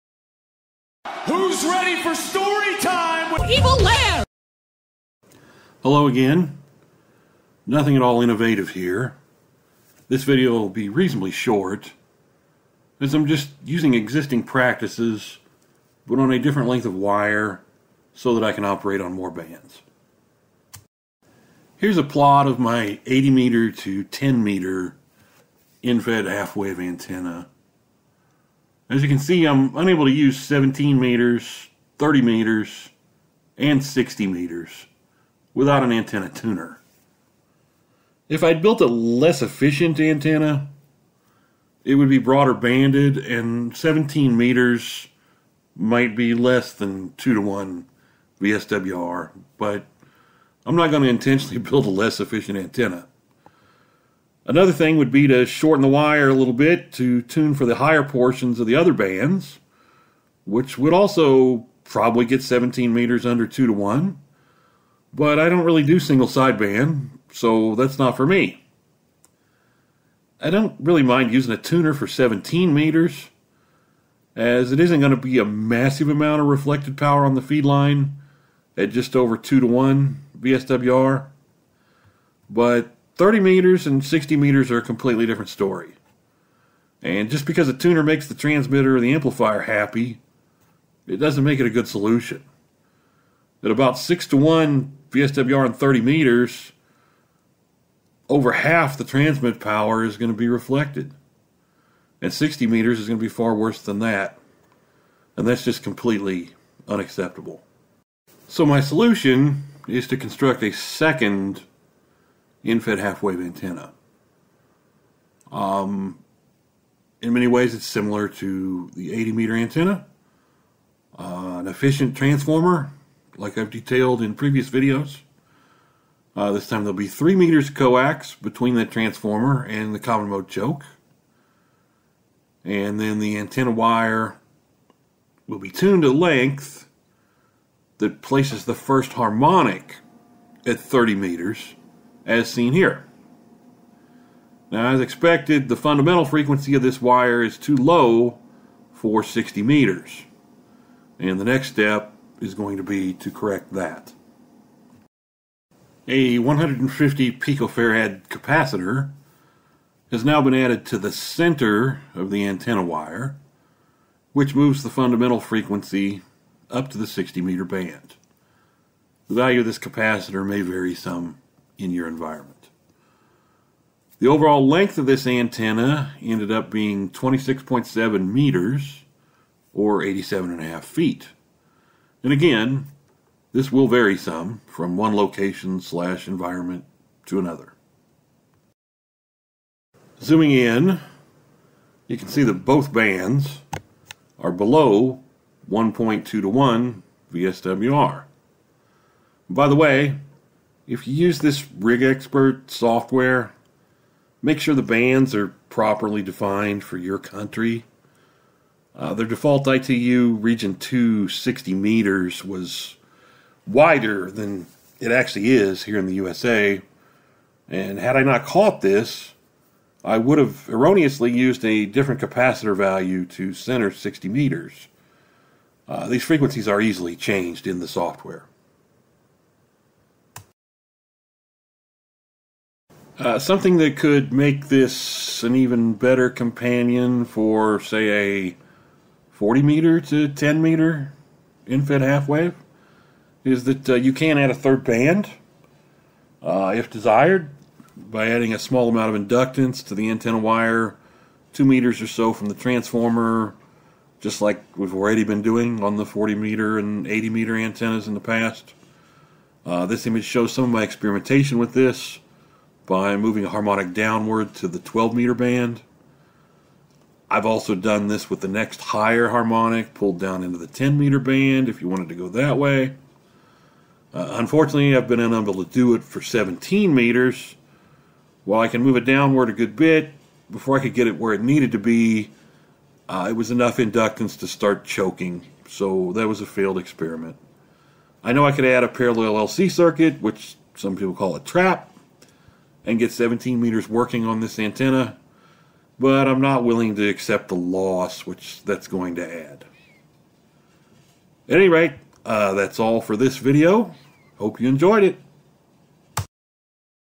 An EFHW antenna for 60 meters & 30 meters, plus 1 more band if desired
It's just using a different length of wire than is commonly used, to make the antenna appropriate for different bands.